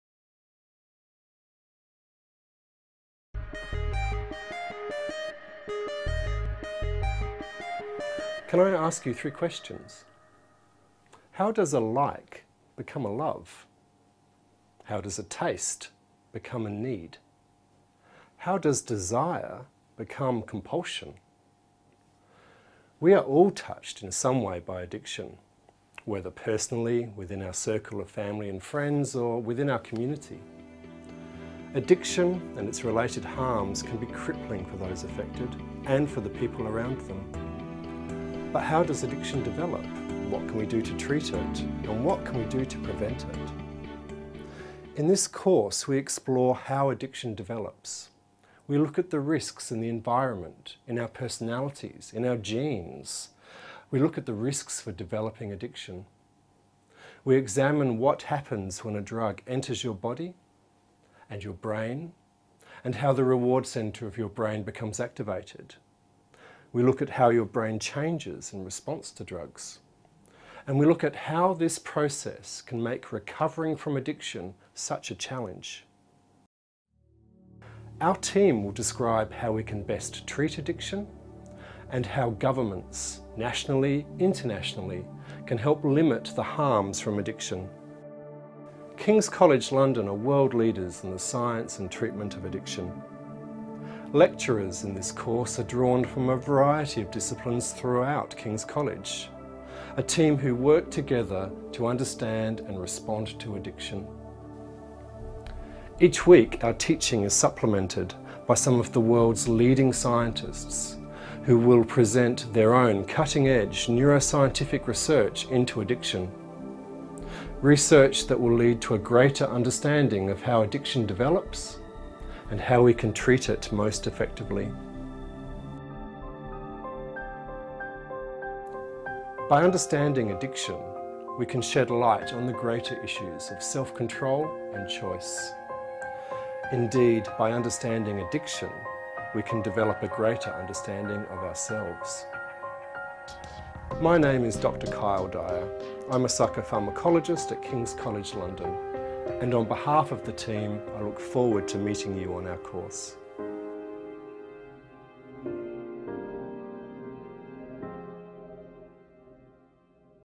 'Understanding Drugs and Addiction' - free online course on FutureLearn.com
Registration for this free online course by King's College London is now closed, browse and sign up for more courses

We are all touched by addiction -- personally, within our circle of family and friends, and within our community. Addiction and its related harms can be crippling for those affected and the people around them. But how does addiction develop? What can we do to treat it? What can we do to prevent it? This course explores how addiction develops, and looks at the risks environmentally, genetically and personally for developing addiction.

The course examines what happens when a drug enters your body and your brain and how your brain changes -- how this process, can make recovering from addiction such a challenge. Our team will describe how we can best treat addiction and how national and international policies help limit the harms from addiction.